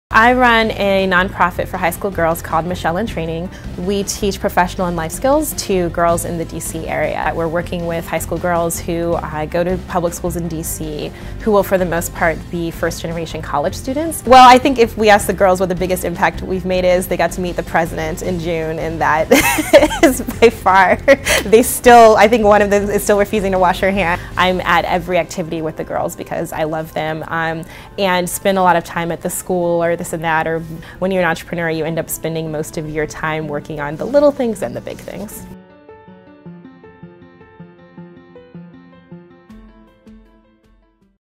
Heroines of Washington 2014 Professional Services Heroine Award: Kat Calvin
The March of Dimes 14th Annual Heroines of Washington® award presentation was held November 18, 2014. The black-tie event honored the significant contributions of women in the D.C. region who exemplify leadership in volunteer community service.

Proceeds support local March of Dimes programs including research, community services, education and advocacy to reduce the occurrence of birth defects, premature birth and infant mortality. Since 2001, the event has raised over $1.7 million to support the March of Dimes mission of saving babies.